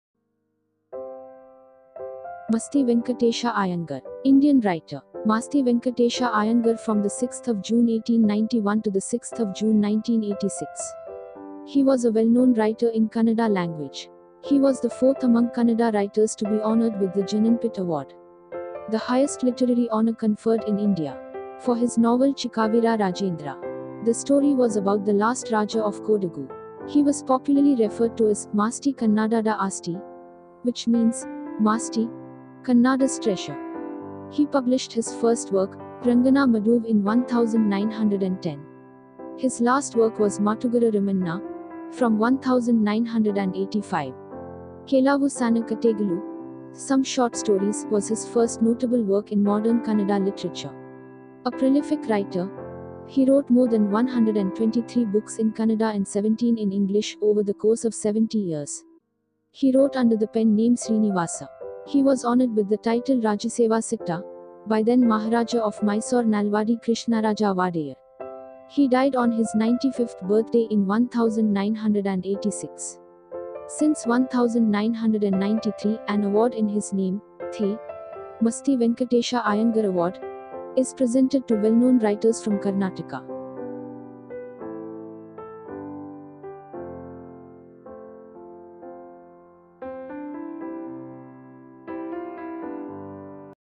Masti Venkatesha Iyengar (6 Jun 1891 – 6 Jun 1986) | Maasti, Kannada's Treasure | Jnanpith Award
He was a well-known writer in Kannada language. He was the fourth among Kannada writers to be honored with the Jnanpith Award, the highest literary honor conferred in India.
He was popularly referred to as Maasti Kannadada Aasti which means "Maasti, Kannada's Treasure". He is most renowned for his short stories. He wrote under the pen name Srinivasa. He was honoured with the title Rajasevasakta by then Maharaja of Mysore Nalvadi Krishnaraja Wadeyar.